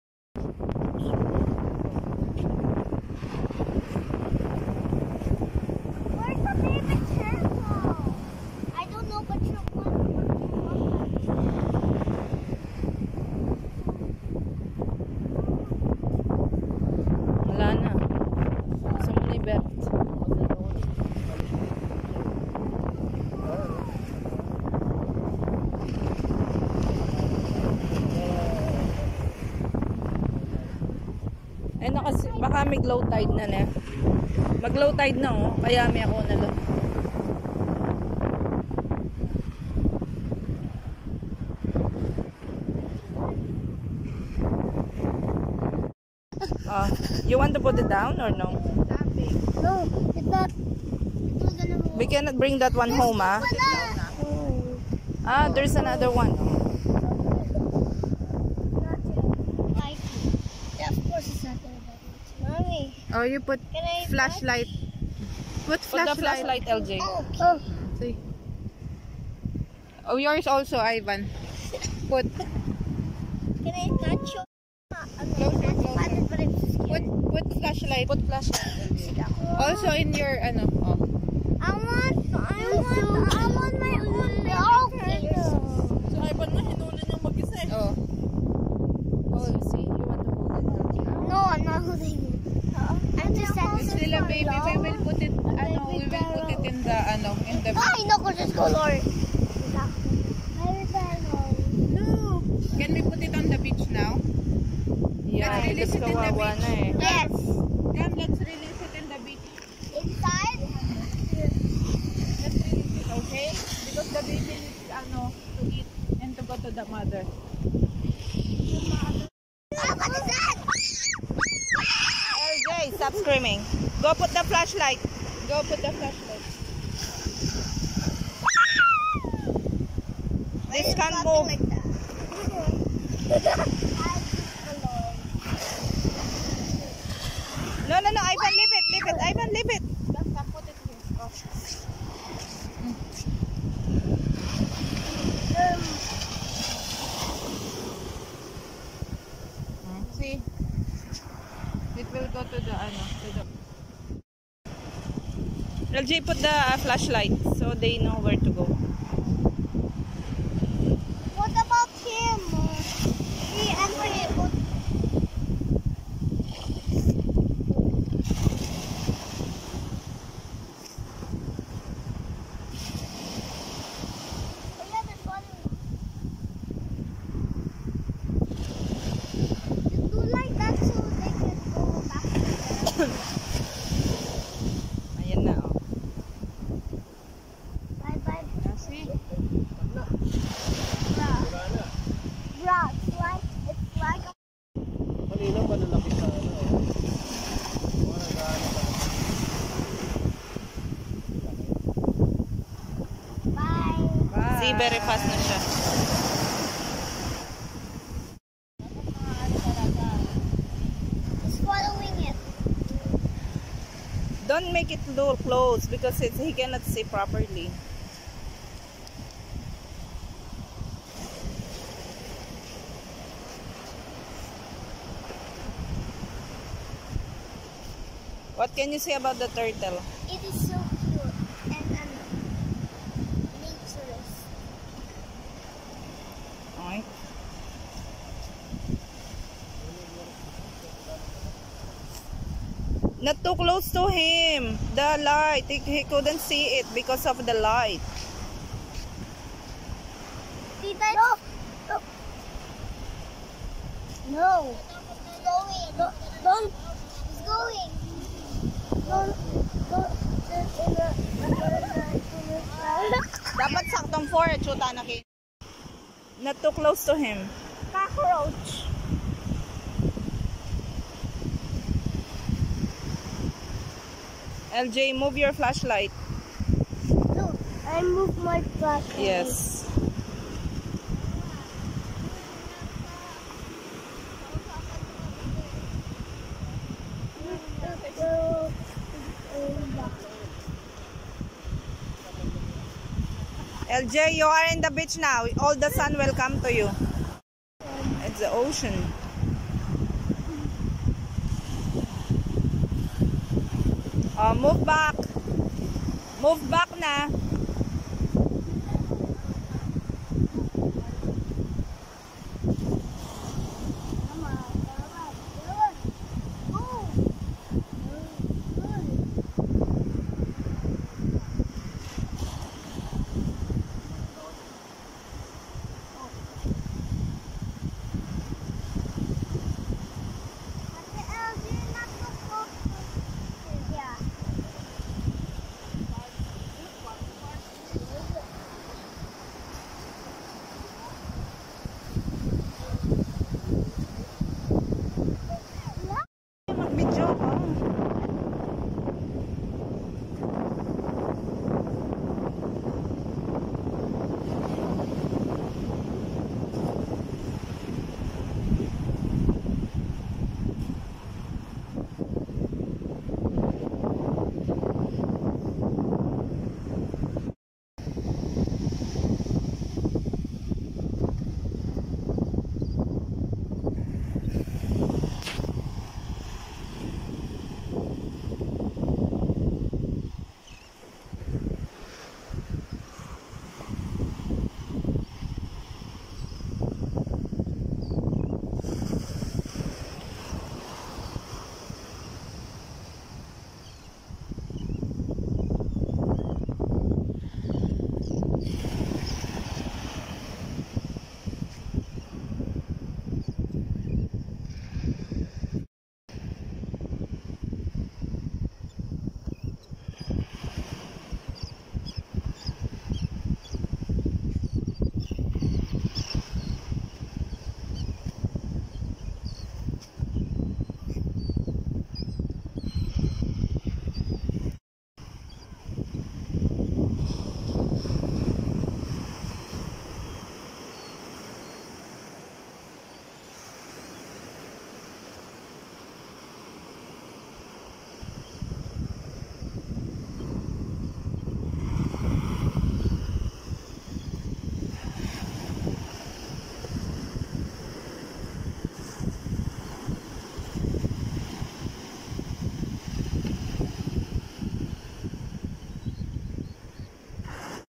TURTLE BEACH/SUR MUSCAT OMAN/FUN WITH KIDS
Turtle beach at Sur Oman, we travel by car from 7PM and we reach in sur by 12 mid night, we book a hotel apartment. And it's cost 30rials for 2 bedroom, seating room and kitchen. Hotel is very clean and approachable attendant. Kid are having fun while walking around the beach and trying to search turtle. We are bit late and found few of small turtle and one big one. Foot steps of baby turtles and big turtle are visible during sunrise. It was a tiring but satisfied adventure.